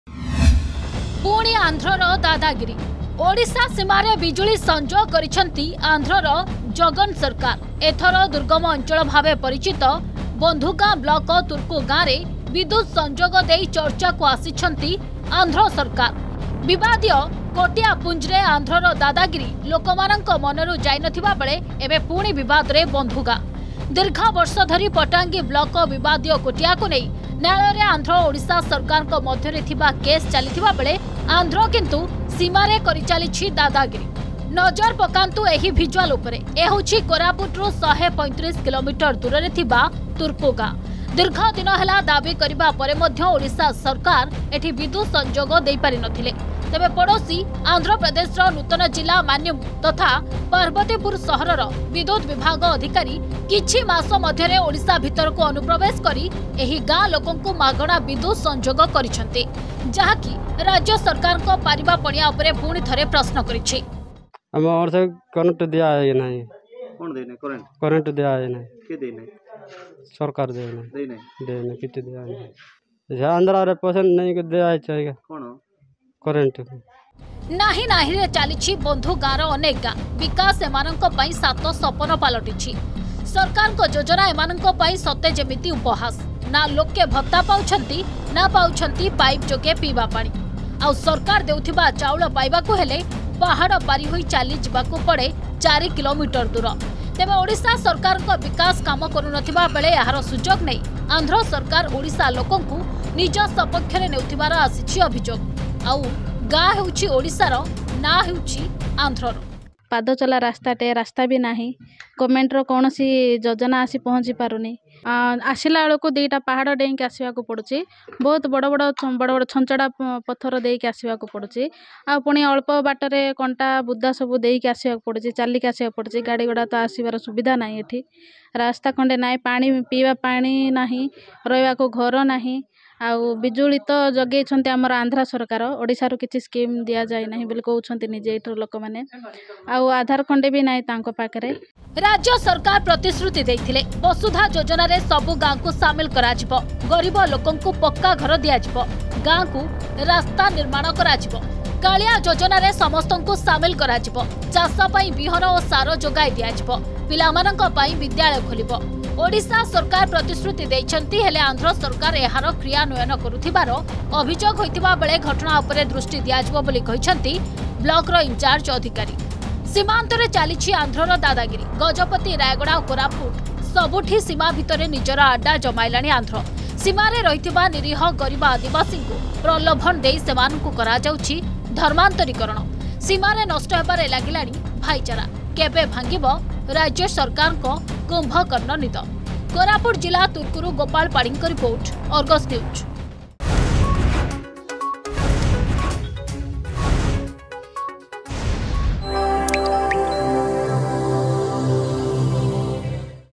ପୁଣି ଆନ୍ଧ୍ରର ଦାଦାଗିରି
ଓଡ଼ିଶାର ଗାଁରେ ଆନ୍ଧ୍ରର ବିଜୁଳି ସଂଯୋଗ । ସୀମାନ୍ତ ଅଞ୍ଚଳରେ ଆନ୍ଧ୍ର ସରକାରଙ୍କ ଯୋଜନା । କହିବାକୁ ଗଲେ ନାଁ ସିନା ଓଡିଶାର ହେଲେ ଗାଁ ପାଲଟିଯାଇଛି ଆନ୍ଧ୍ର ସରକାରଙ୍କର । ରାଜ୍ୟ ସରକାରଙ୍କ ଦୁରଦୃଷ୍ଟି ଅଭାବ ପାଇଁ କୋଟିଆପୁଞ୍ଜ ପରେ ଏବେ ବନ୍ଧୁଗାଁର ତୁରୁକୁ ଉପରେ ପଡ଼ିଛି ଆନ୍ଧ୍ରର ଲୋଲୁପ ଦୃଷ୍ଟି । ଦେଖିବା ଅର୍ଗସର ଏକ୍ଲକ୍ଲୁସିଭ ରିପୋର୍ଟ